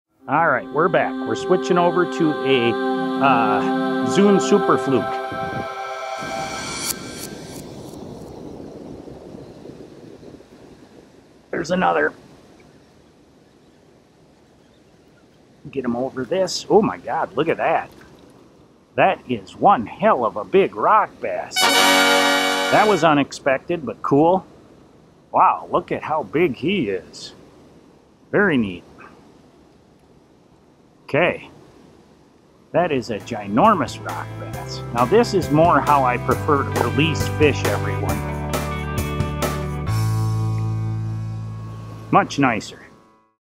Fish of the week! A ROCK BASS AS BIG AS YOUR HAND! April 10, 2024
Fish of the week!  This week I caught a rock bass as big as your hand!